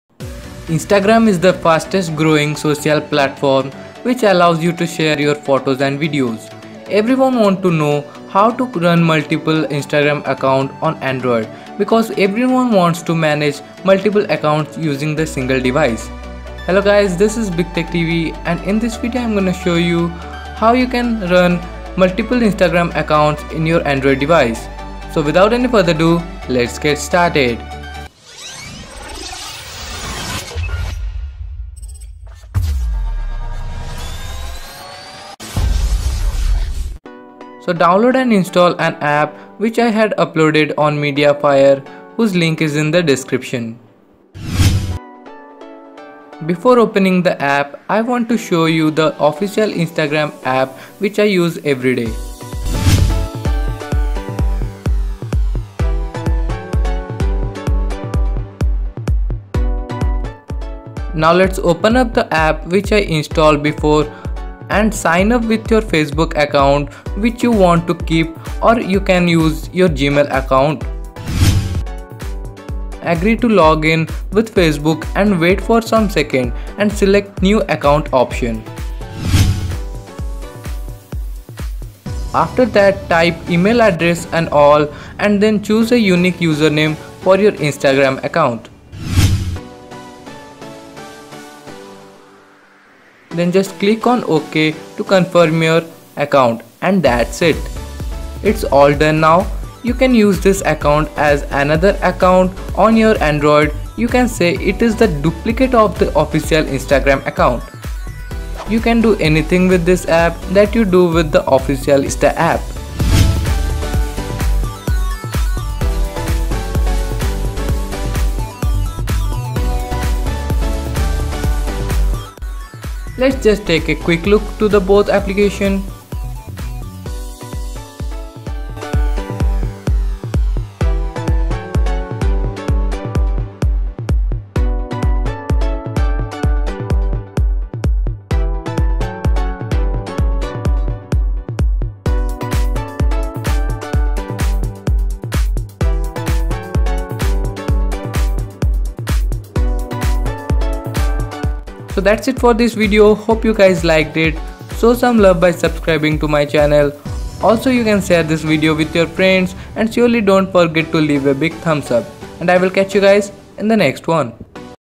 Multiple INSTAGRAM accounts on a Android [NO-ROOT]
Instagram is the fastest growing social platform which allows you to share your photos and videos. Everyone wants to know  how to run multiple Instagram accounts on android because everyone wants to manage multiple accounts using the single device. So in this video i am gonna show u how you can run multiple instagram accounts in your android device.So without any further do lets get started.

Thank u
Manas :)

Please watch New Video : "Divoom Timebox Mini : Best Desk accessory of 2017? LED Speaker"